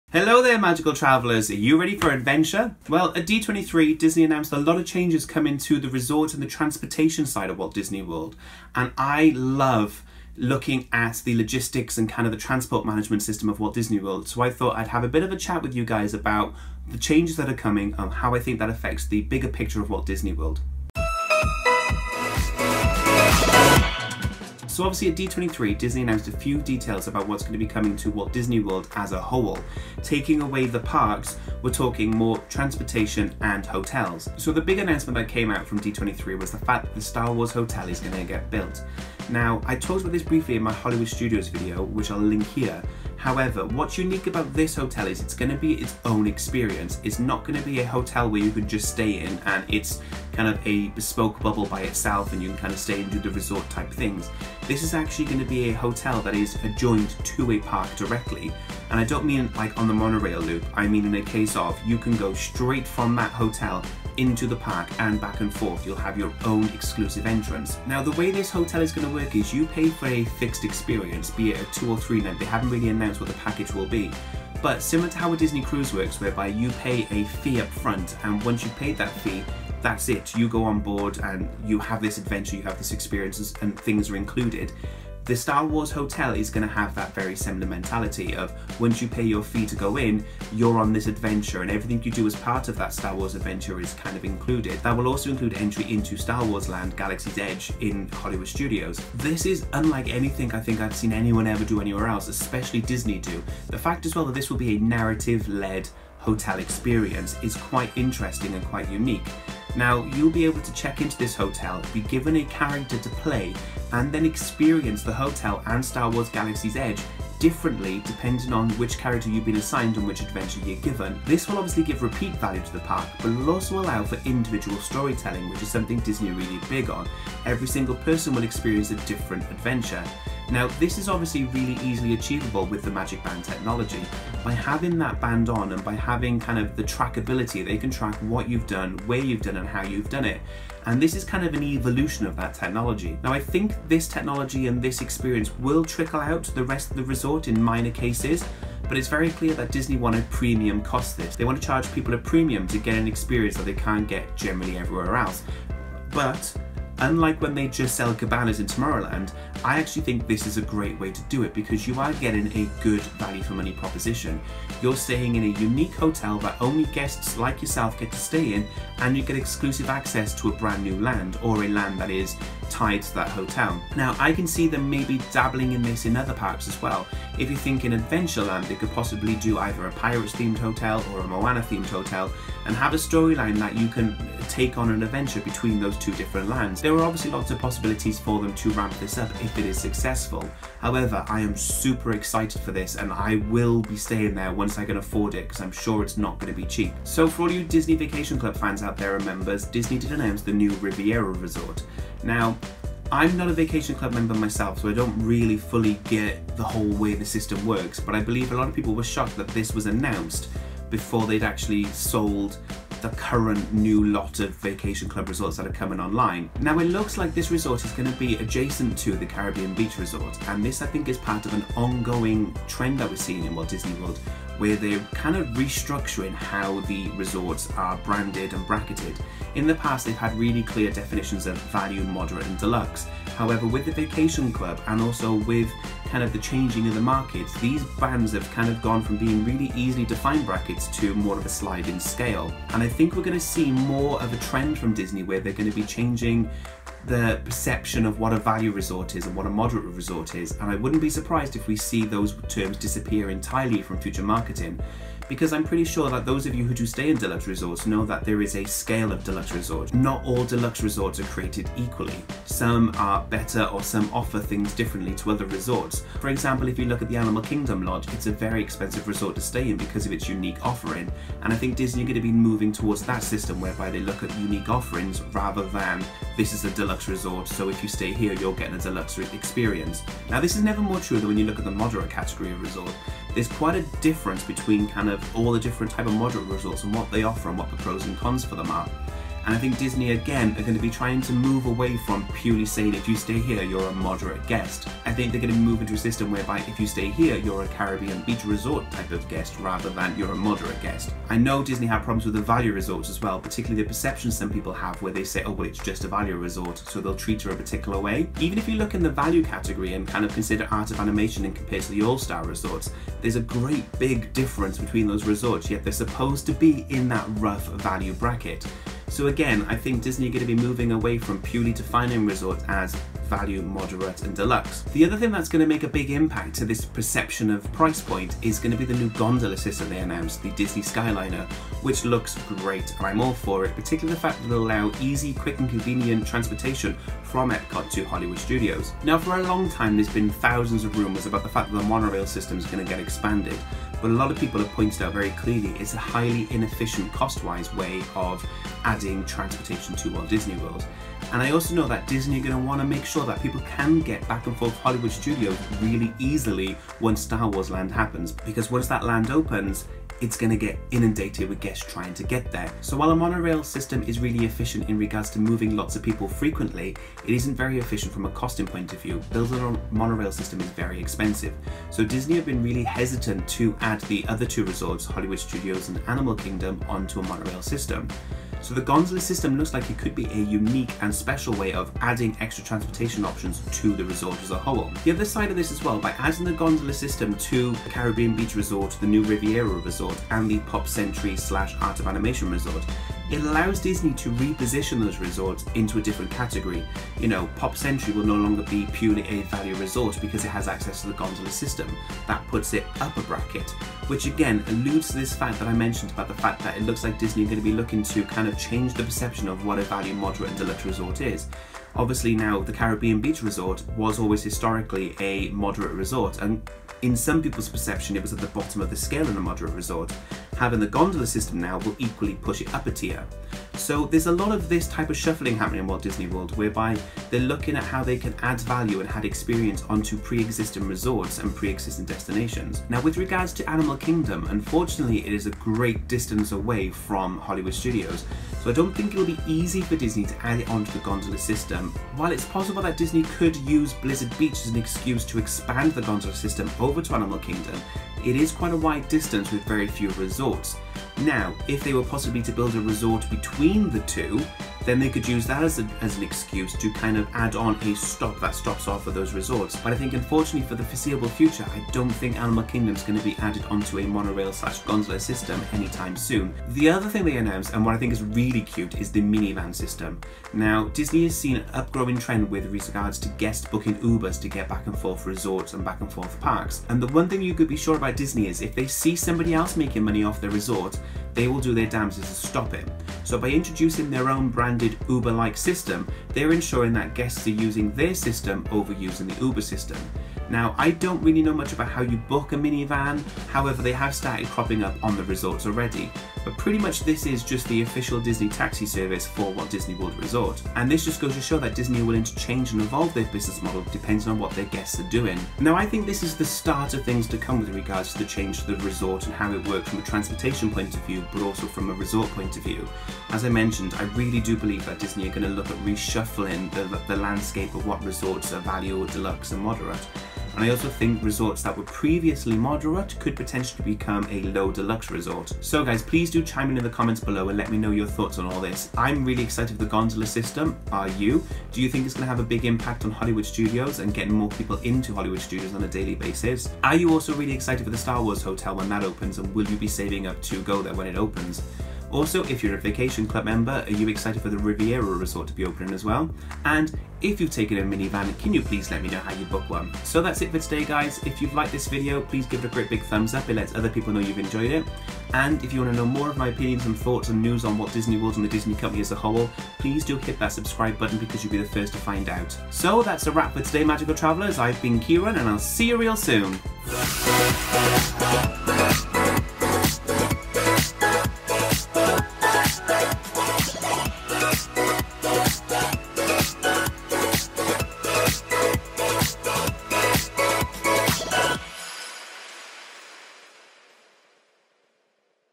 New Transport and Resorts coming to Walt Disney World - Star Wars Hotel, Gondolas and Disney Uber
D23 had some big announcements for the whole of Walt Disney World, especially for the resorts at Walt Disney World.

A whole new way to get to and from Epcot and Hollywood studios is on its way, as well as a whole new Disney Vacation Club resort near both Epcot and Hollywood Studios.

Disney take on Uber, and add Gondola's.

Hollywood Studios gets its own exclusive Star Wars themed Hotel